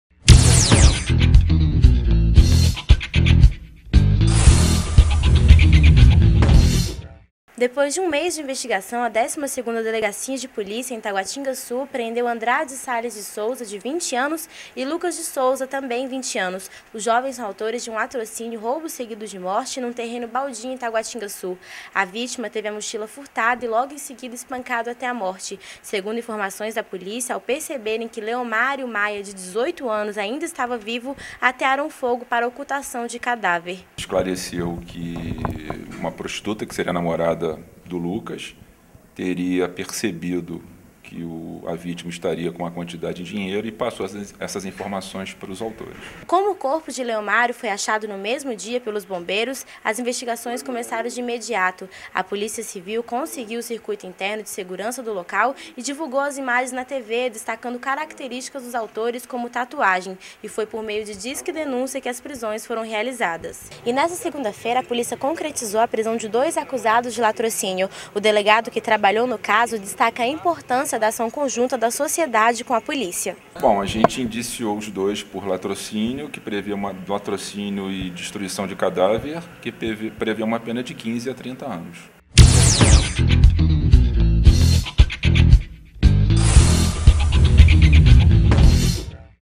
LATROCÍNIO - 19/07/2011
Depois de roubarem atearam fogo na vítima